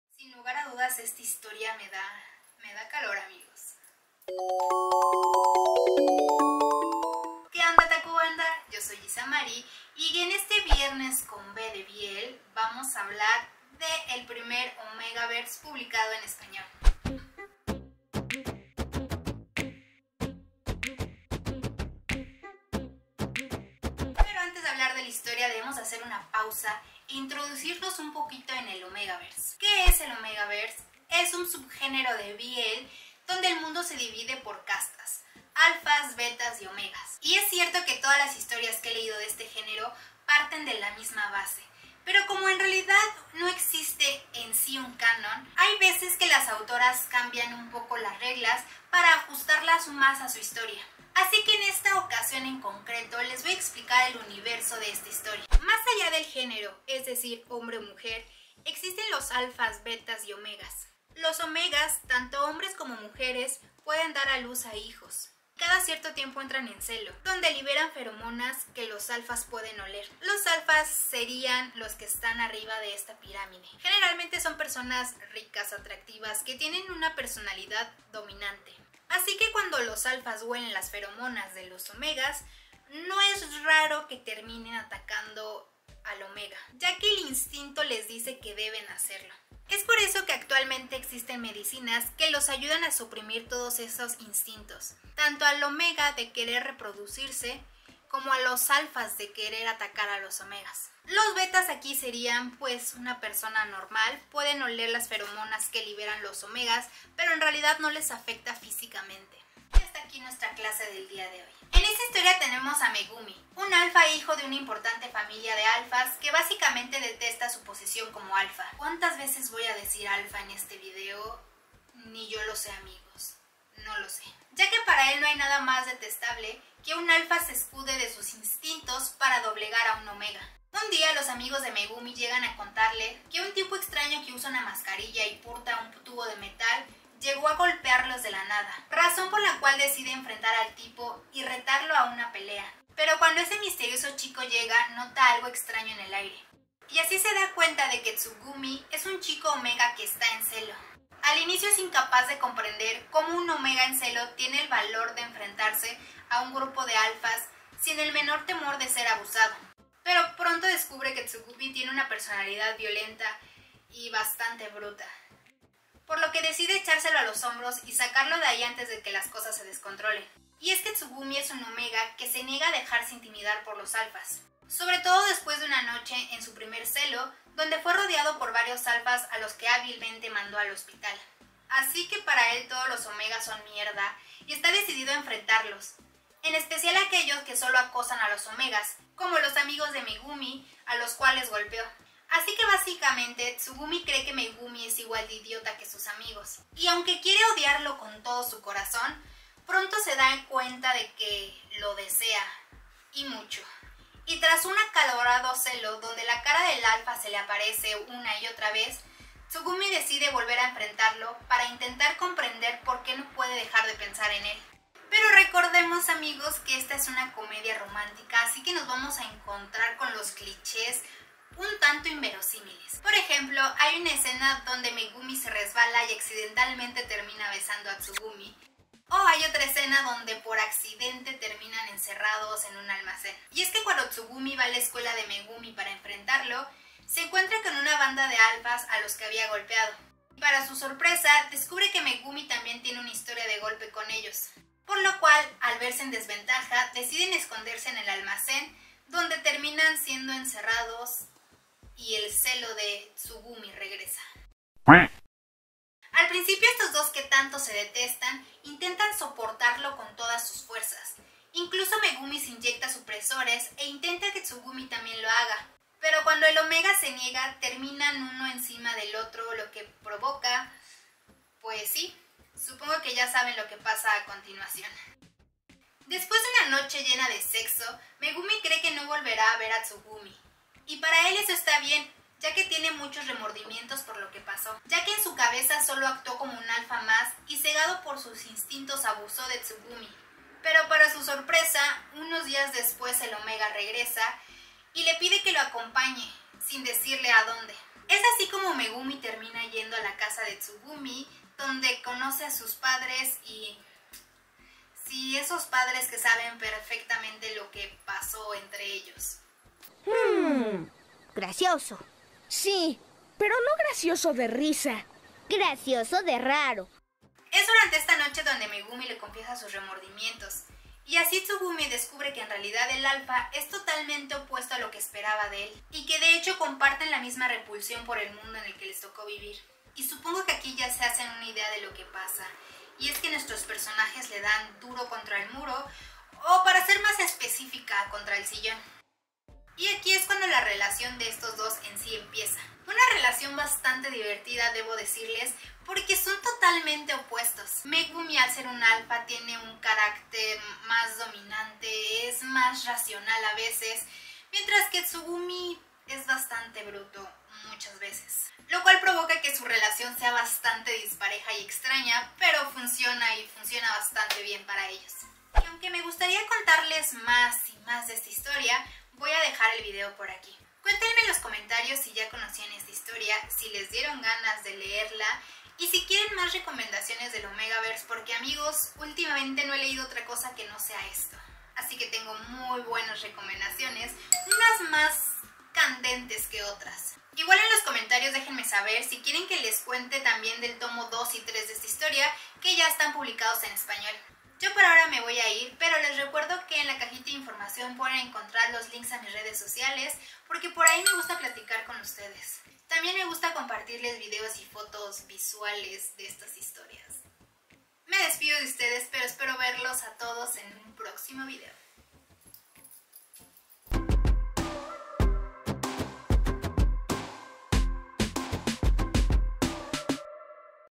¿Qué es el Omegaverse? / Megumi y Tsugumi 💕
¡Hola, otaku banda!
ಡ ͜ ʖ ಡ

El día de hoy vamos hablar del primer Omegaverse publicado en español, «Megumi y Tsugumi », un manga divertido y cómico que sé que les va a gustar.

💕 ¡VAMOS A SEGUIRNOS! 💕